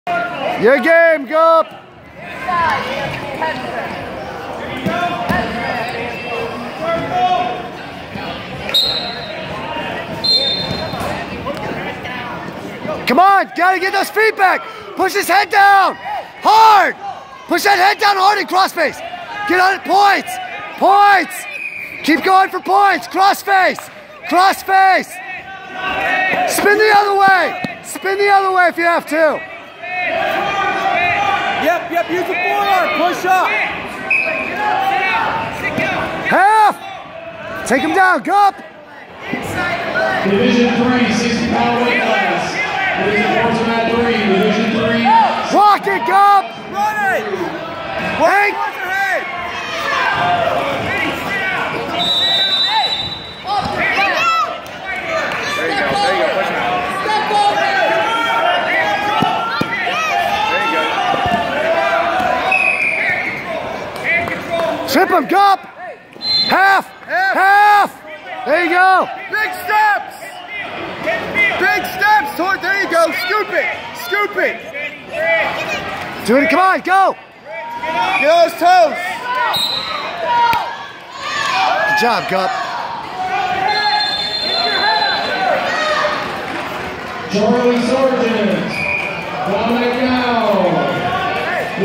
Guppie Pinning At Shelby
Jakob Gibson, Garage Rats, wrestling and pinning at Shelby 2/4/17